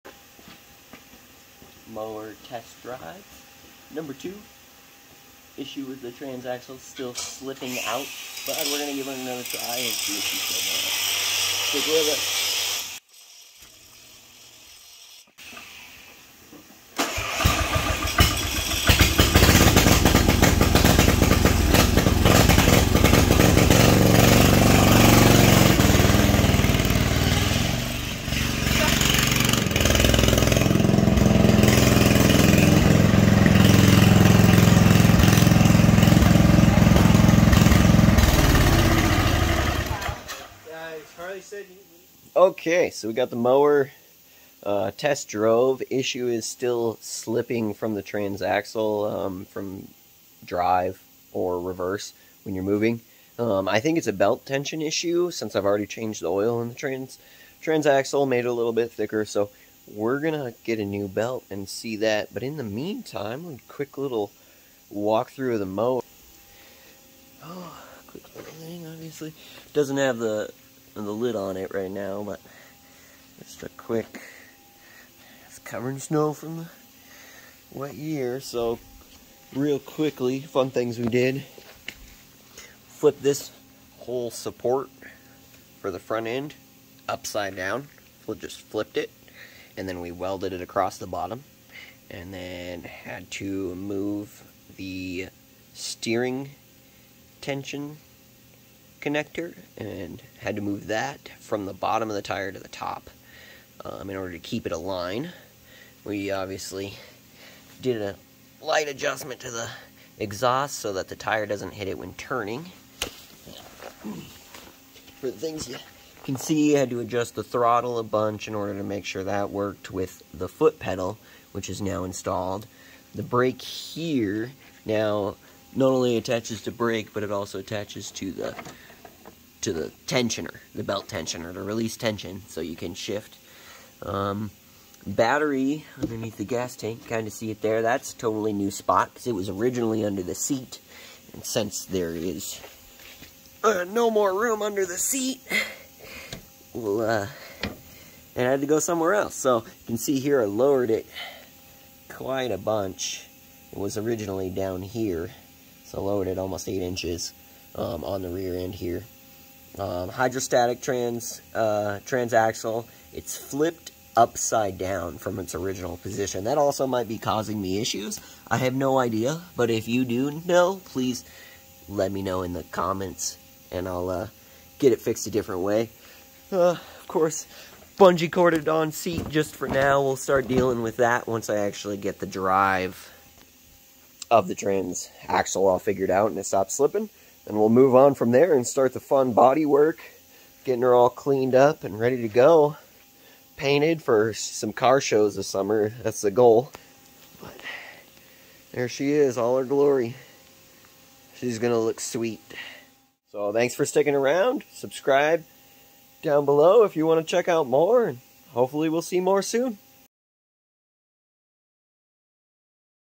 Racemower Build Test 2! Walk through on fabrication/Test drive
Here is a bit on how I made the custom race mower! lots of work at this point to do but it is coming together really well!

Lawn mower performance mods